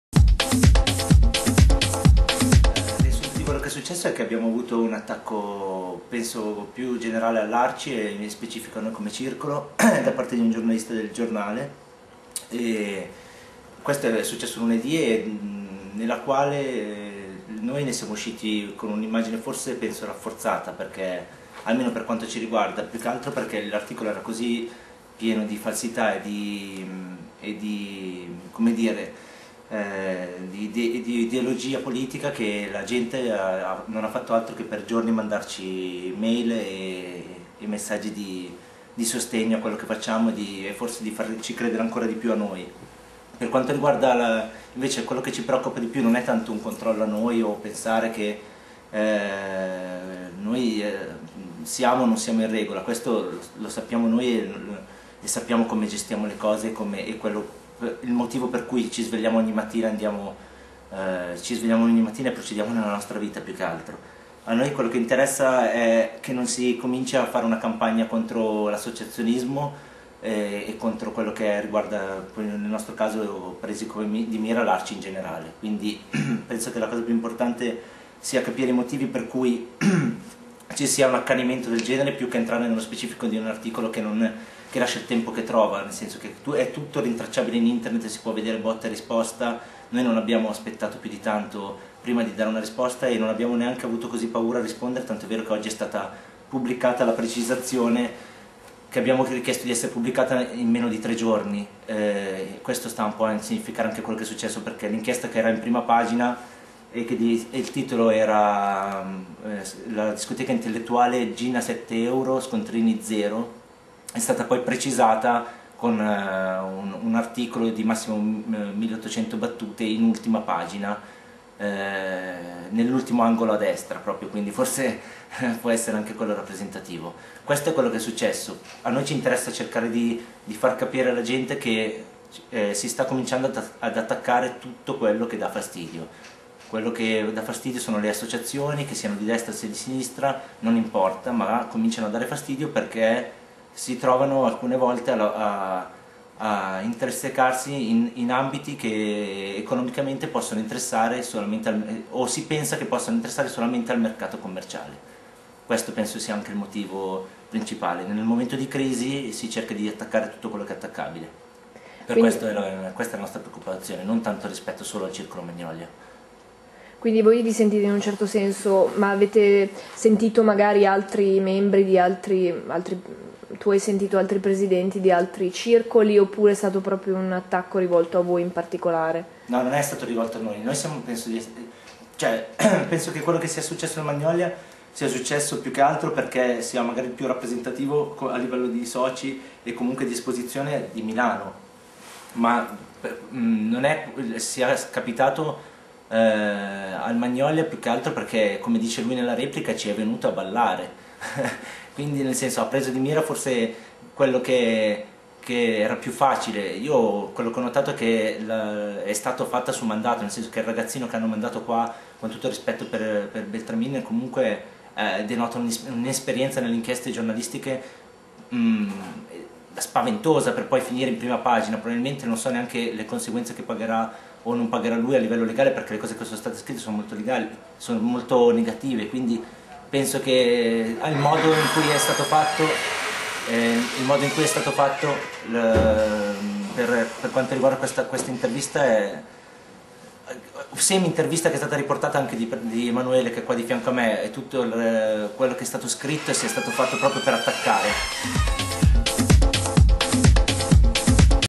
Il circolo Magnolia risponde a Il Giornale - 1
In un articolo apparso su Il Giornale è stato attaccato il circolo Arci Magnolia di Segrate. Li abbiamo raggiunti.

intervista Arianna Ascione